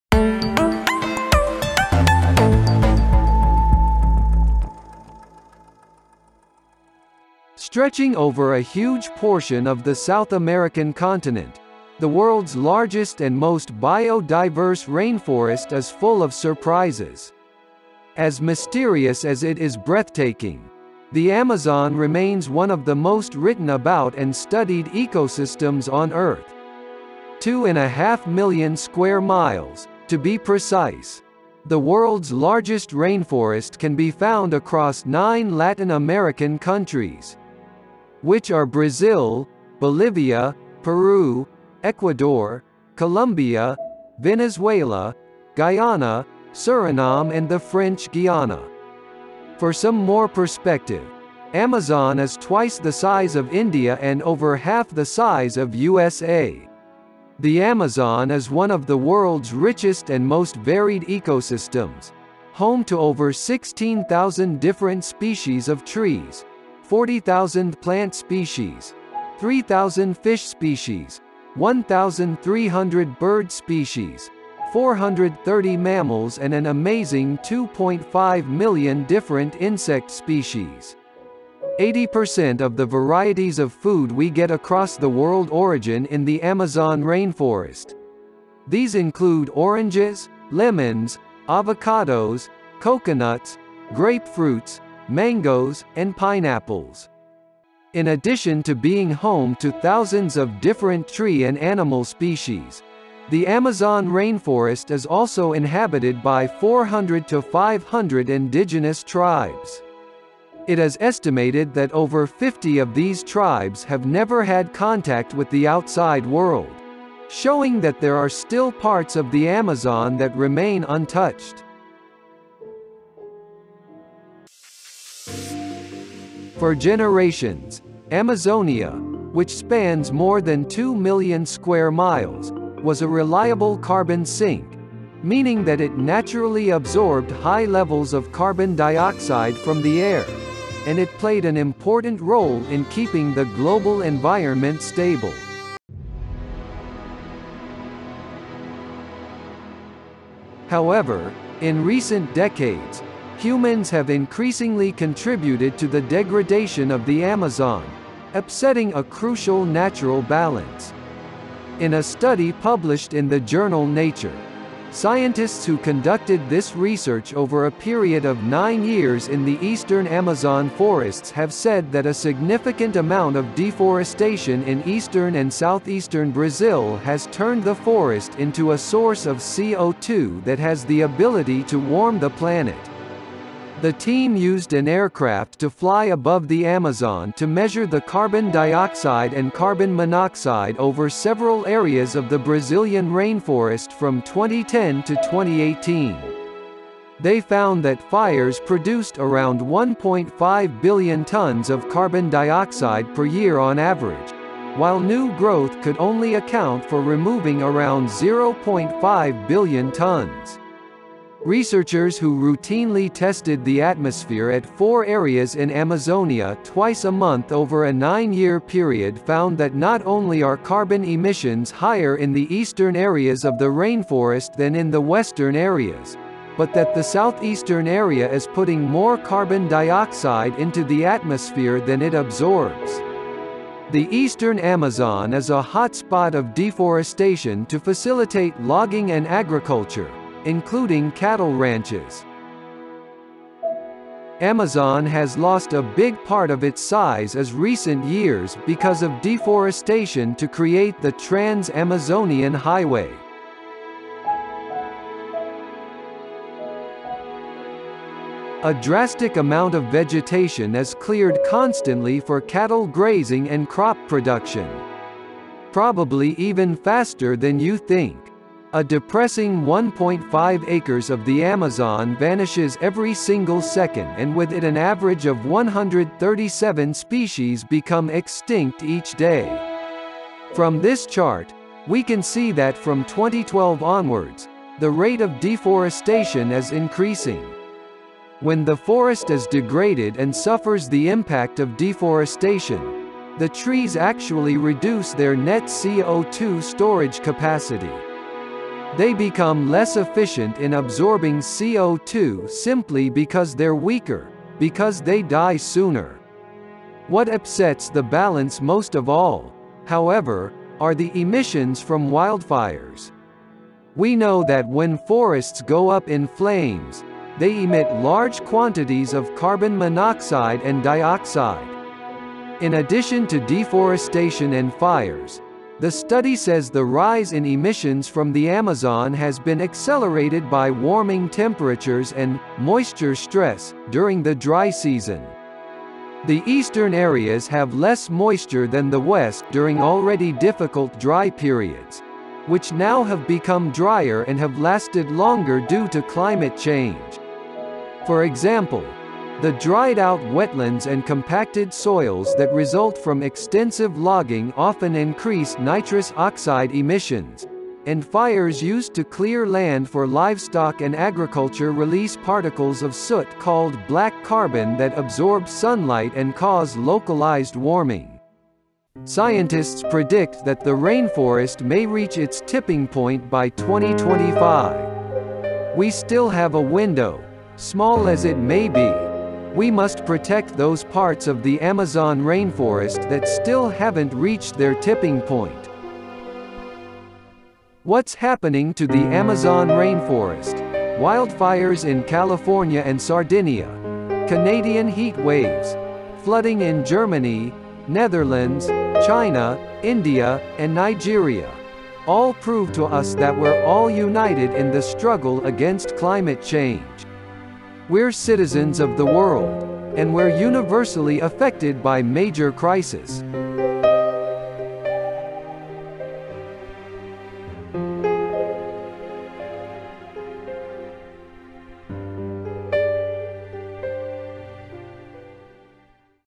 This is why The Amazon Rainforest now releasing more carbon than it absorbs
A newly published study shows that parts of the Amazon rainforest are emitting more carbon dioxide than they absorb.

Findings from the nearly decade-long research project, published in the journal Nature, suggest that deforestation and fire, among other factors, have dramatically undercut the Amazon's ability to absorb heat-trapping carbon emissions from the atmosphere.

Researchers who routinely tested the atmosphere at four areas in Amazonia twice a month over a nine-year period found that not only are carbon emissions higher in the eastern areas of the rainforest than in the western areas, but that the southeastern area is putting more carbon dioxide into the atmosphere than it absorbs. The eastern Amazon is a hotspot of deforestation to facilitate logging and agriculture, including cattle ranches.

The most deforested areas of the Amazon had drier, warmer and longer dry seasons, the study found. Dry months saw a temperature increase of 2 degrees Celsius in these parts of the Amazon, increasing the forest’s flammability and restricting its ability to absorb carbon dioxide.